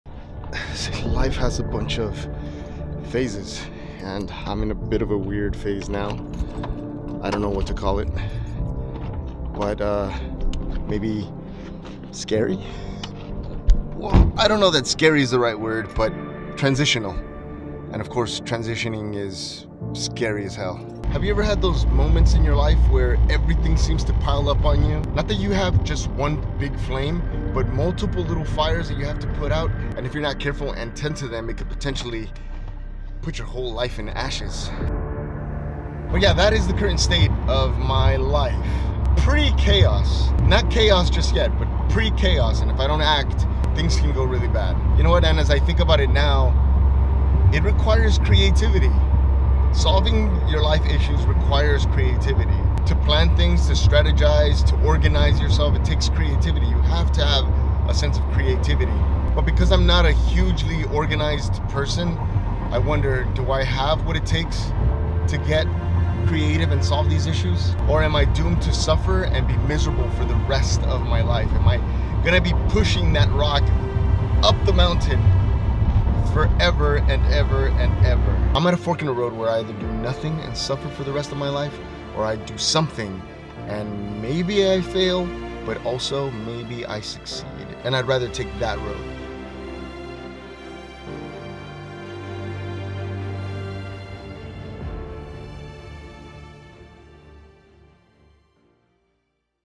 FIRES EVERYWHERE // STOIC MAKER // VLOG 001
There are many phases in life, I am in a current phase where if I don't tend to the little fires things can get out of control fast. How do you approach your personal life fires?

SC: 598